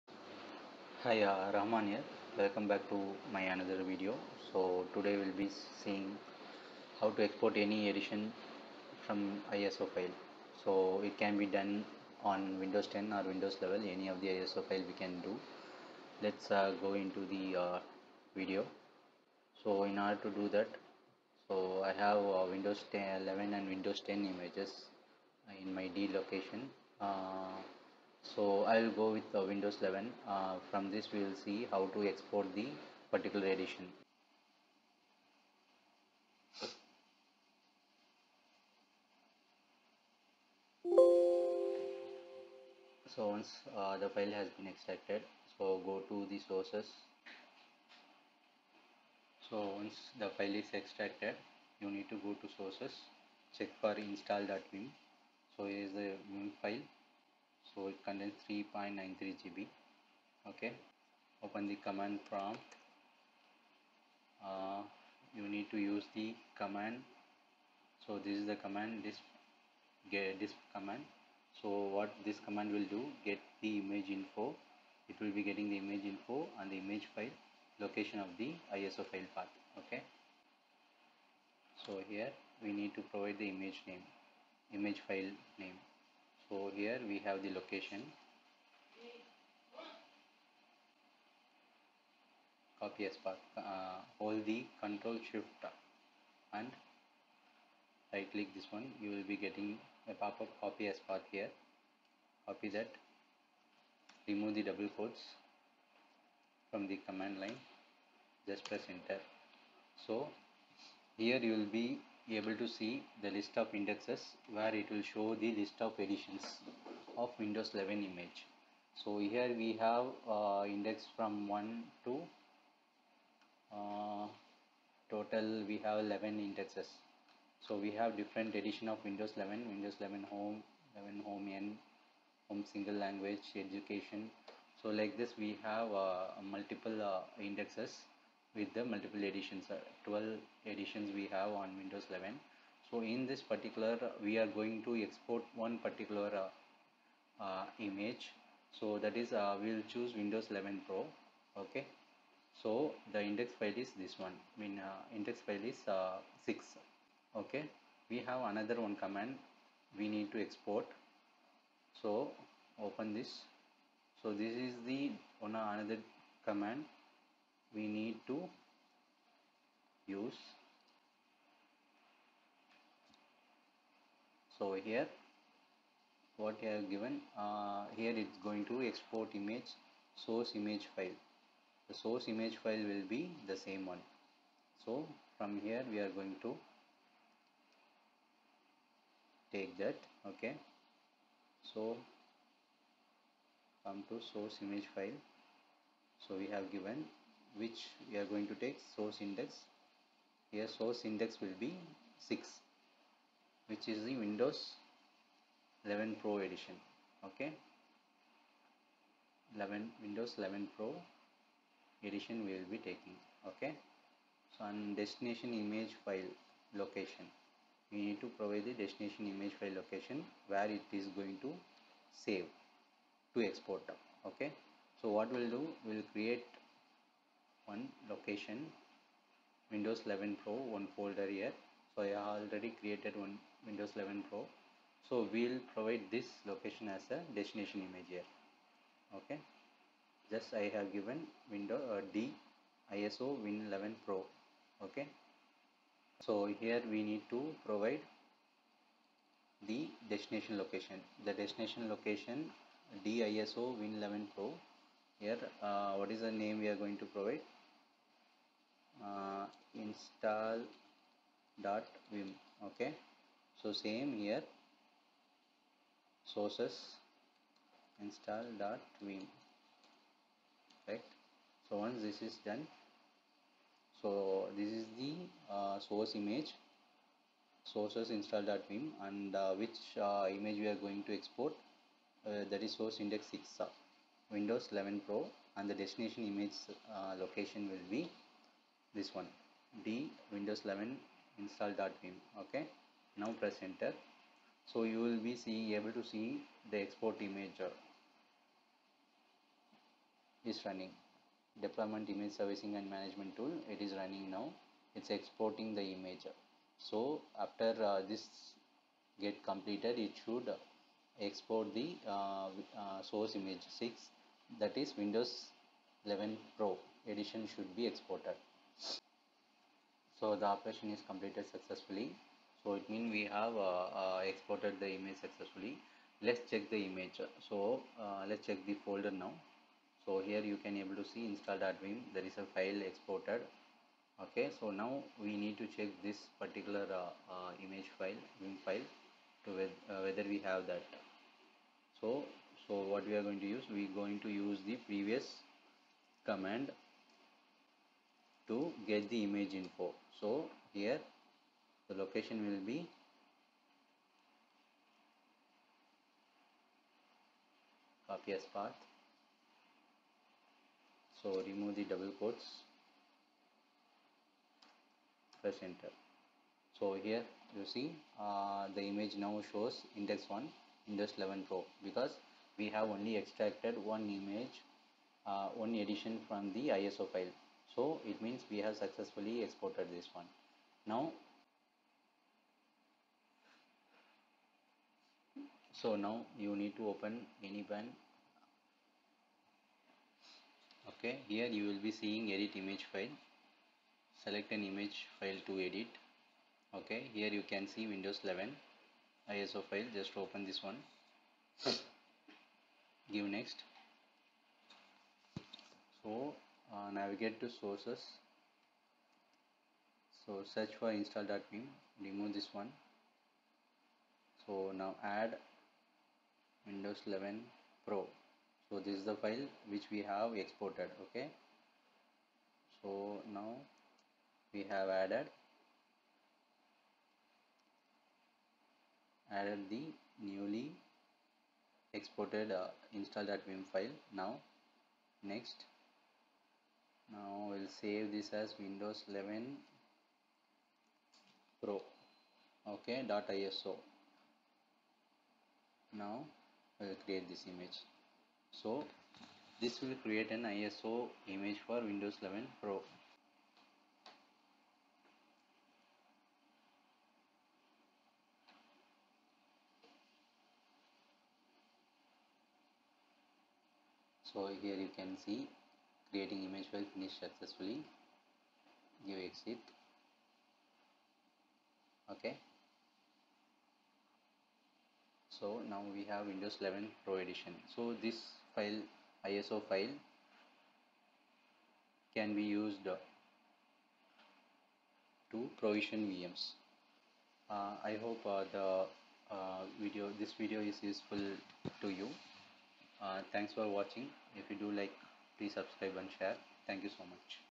Export any edition of Window 11 from ISO image
Hi Friends,
In this tutorial, I will show you how to Export Any Edition of Window 11 from ISO image.

𝘾𝙤𝙢𝙢𝙖𝙣𝙙 𝙩𝙤 𝙘𝙝𝙚𝙘𝙠 𝙒𝙞𝙣𝙙𝙤𝙬𝙨 𝙫𝙚𝙧𝙨𝙞𝙤𝙣 𝙞𝙣𝙨𝙞𝙙𝙚 𝙄𝙎𝙊
Dism /Get-ImageInfo /ImageFile:(ISO File Path)

𝘾𝙤𝙢𝙢𝙖𝙣𝙙 𝙩𝙤 𝙨𝙚𝙥𝙖𝙧𝙖𝙩𝙚 𝙖 𝙨𝙞𝙣𝙜𝙡𝙚 𝙊𝙎 𝙛𝙧𝙤𝙢 𝙖 𝙢𝙪𝙡𝙩𝙞-𝙫𝙚𝙧𝙨𝙞𝙤𝙣 𝙄𝙎𝙊
Dism /Export-Image /SourceImageFile: /SourceIndex: /DestinationImageFile: /Compress:max /CheckIntegrity